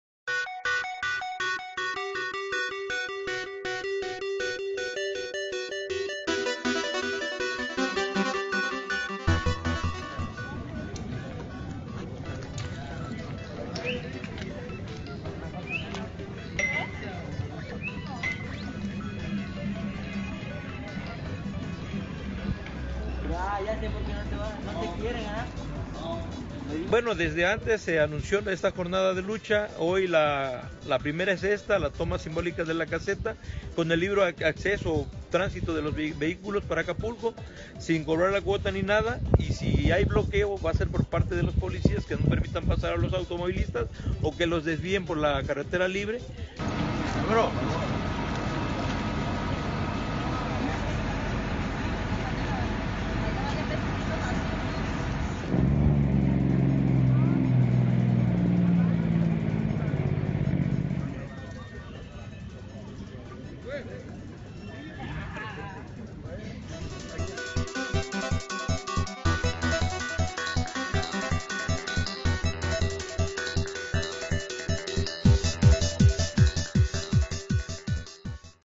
Toman normalistas y padres de los 43 desaparecidos la caseta de Palo Blanco
Los estudiantes de Ayotzinapa y los familiares de los desaparecidos en Iguala hace casi dos años tomaron la garita pero no detuvieron el tráfico y sólo dieron el libre paso a los automovilistas. Anunciaron que esta es la primera de las acciones que se emprenderán para la conmemoración de los sucesos. Al sitio llegaron policías estatales.